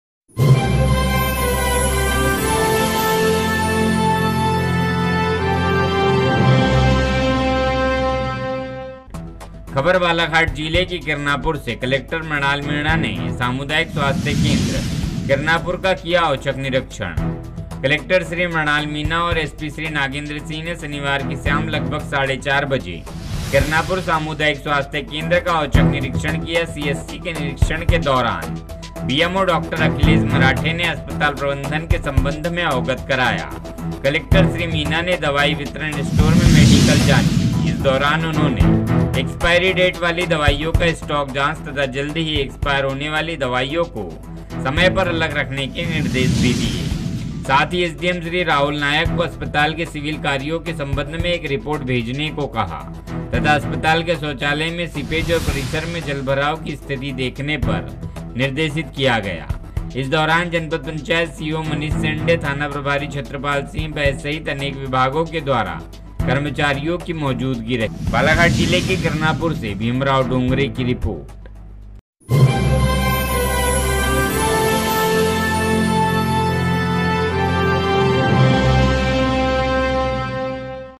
कलेक्टर मृणाल मीणा ने सामुदायिक स्वास्थ्य केन्द्र किरनापुर का किया औचक निरीक्षण
नमस्कार दोस्तों,